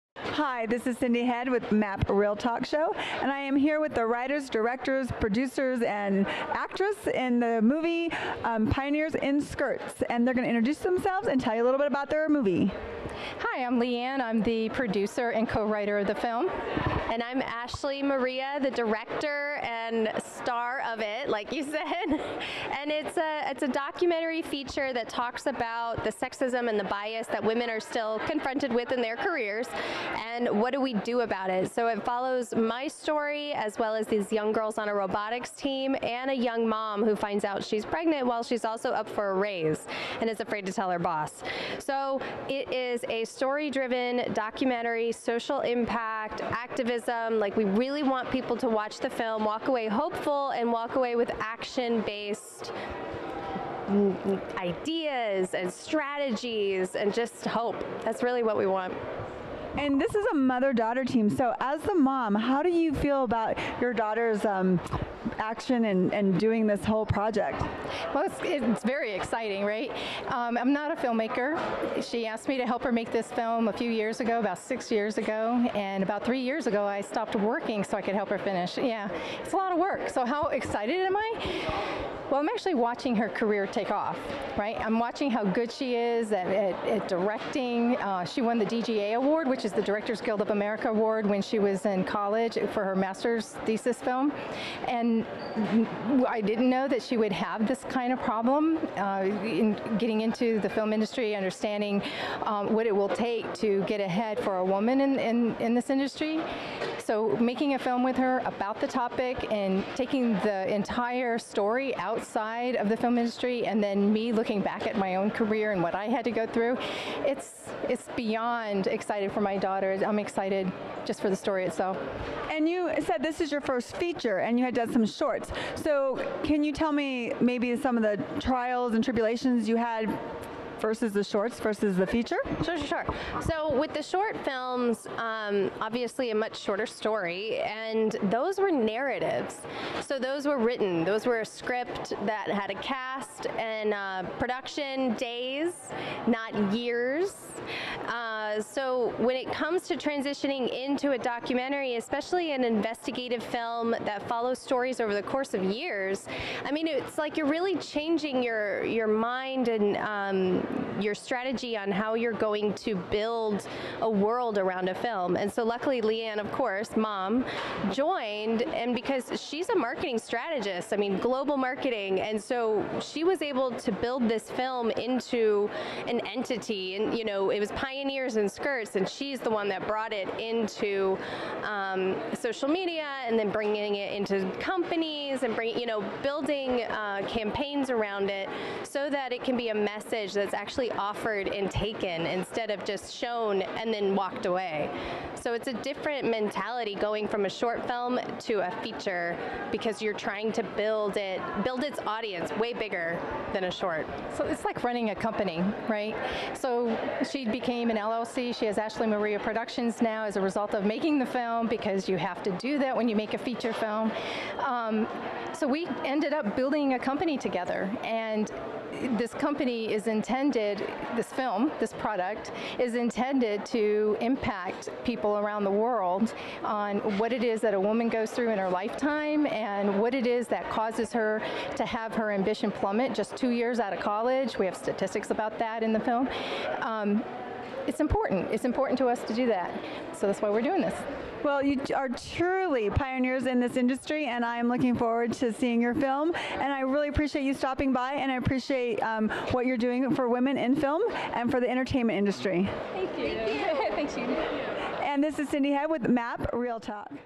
Worldwide Women's Film Festival - Pioneers in Skirts
Check out this interview snippet with the Pioneers in Skirts team and Cyndee Head of the MAAP Real Talk Show. The film won "Best Documentary Feature" at the 2020 Worldwide Women's Film Festival!